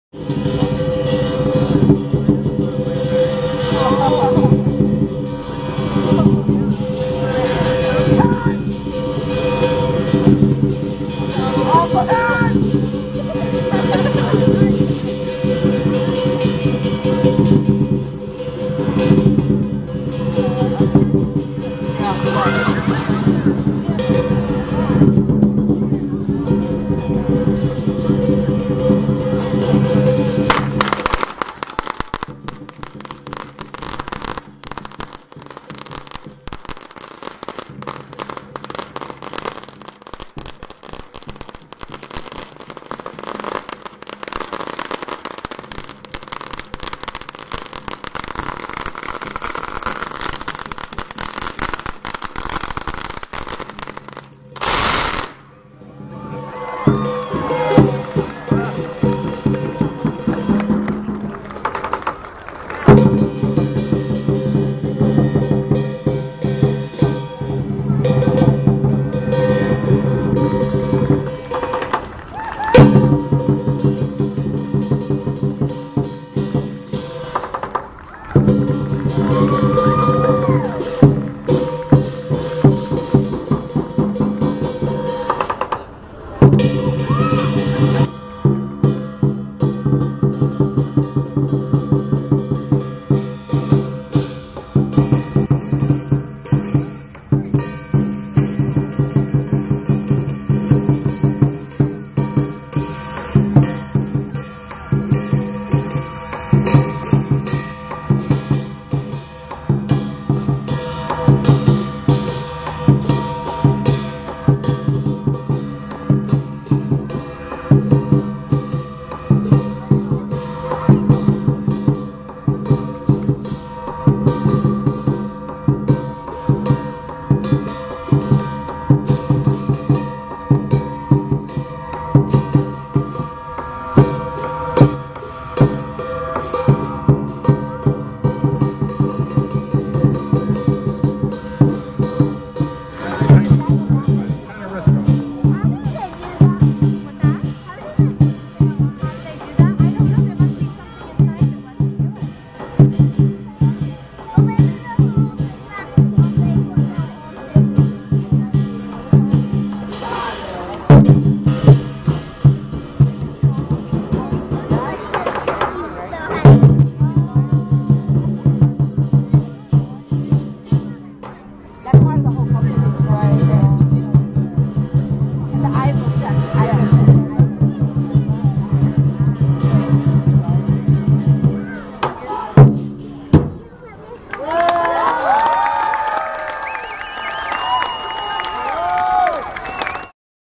dragons
Chinese Dragons during Beachcomber Days Parade, Waldport, OR, June 15, 2013